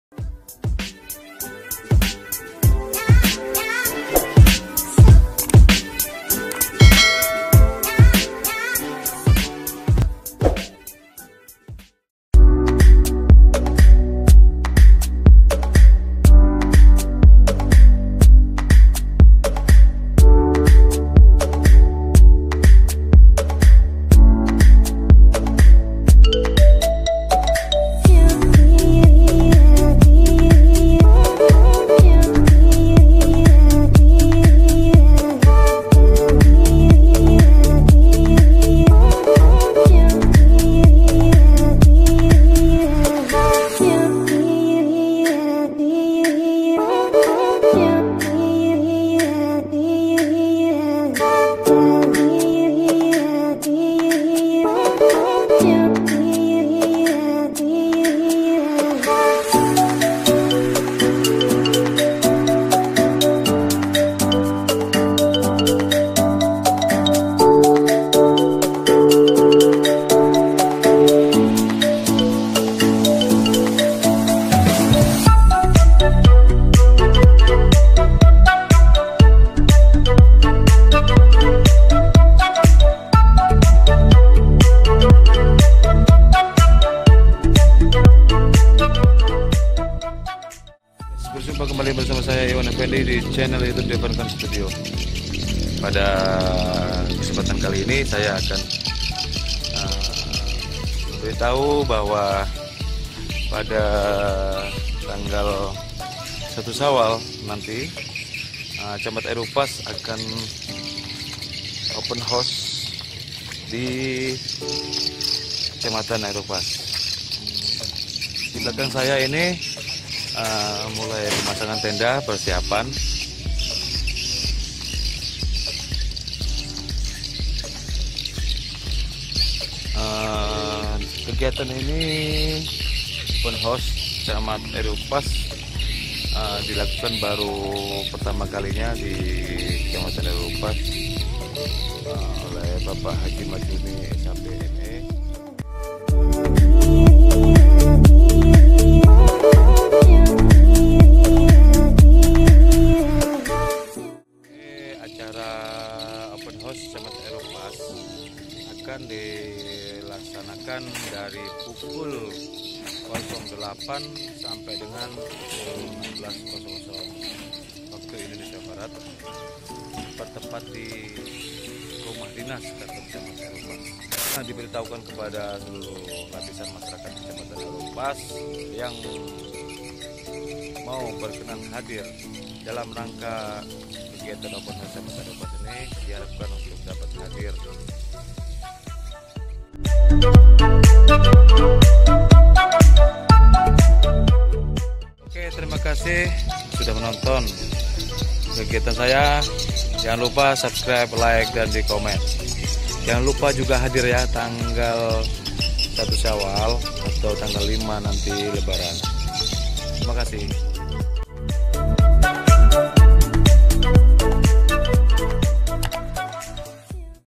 PERSIAPAN OPEN HOUSE CAMAT AIR UPAS LEBARAN 1440 H DI RUMAH DINAS AIR UPAS
Air upas, 4 Juni 2019, kami sedang mempersiapkan untuk Open House Camat Air Upas, dalam rangka Hari raya Idul Fitri 1440 H/2019 M, jangan lupa datang ya tanggal 5 Juni 2019 1 Syawal 1440 H. Terbuka Untuk Siapa Saja. kegiatan ini baru pertama kali di adakan di Kantor Camat Air Upas.